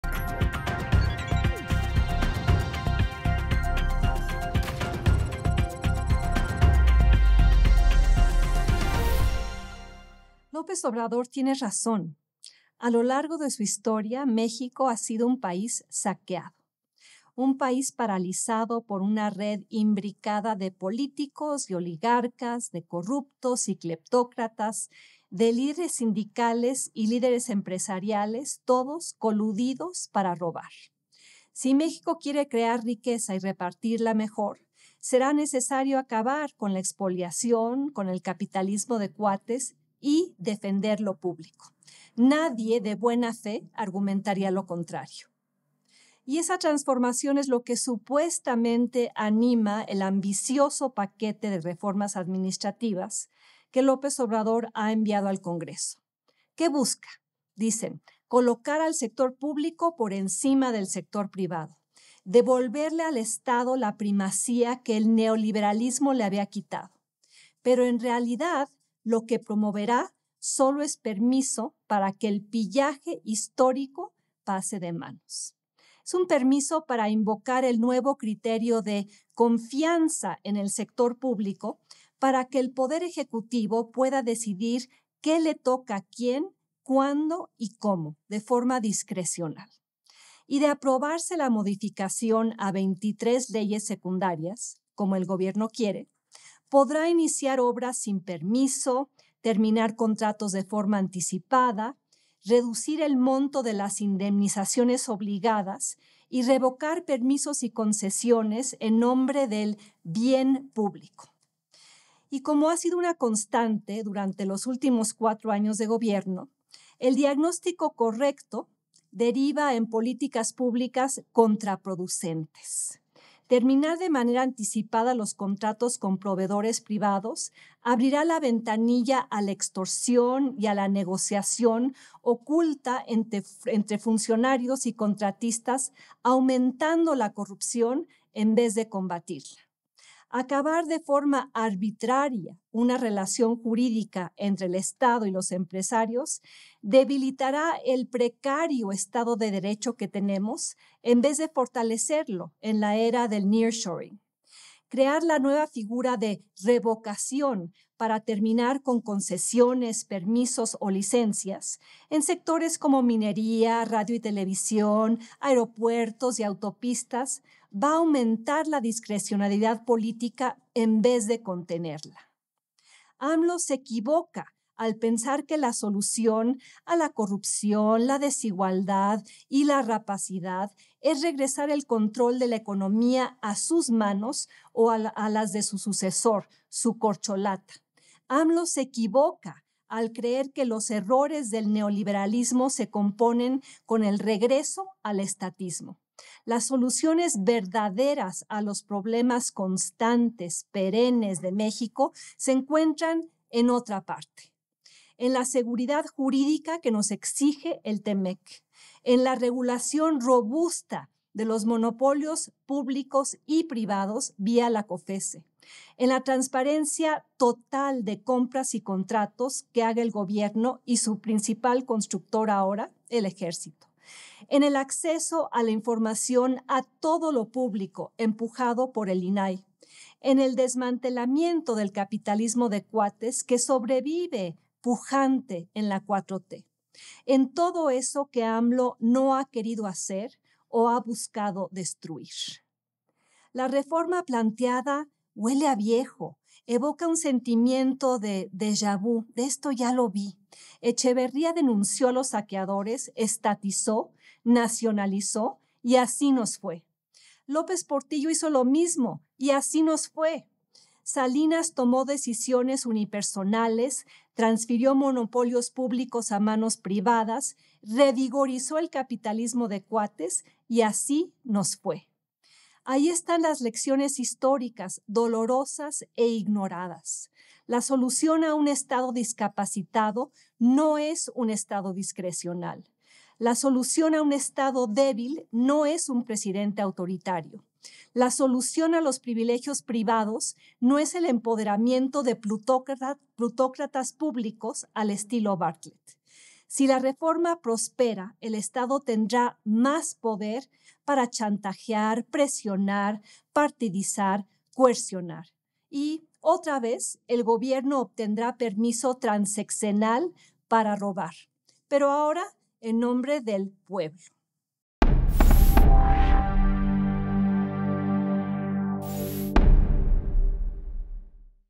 La reforma administrativa de AMLO es un permiso para que el saqueo pase a la 4T: Denise Dresser
Denise Dresser comparte el diagnóstico del presidente Andrés Manuel López Obrador en el sentido de lo mucho que ha sido saqueado México.